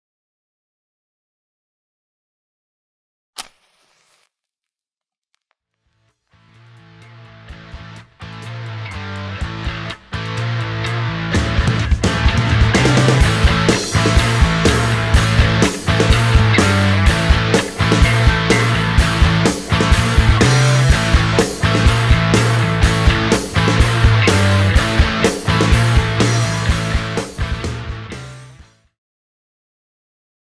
Gatuzz - Debut album (Teaser trailer #1)
First teaser trailer for the debut album by Gatuzz.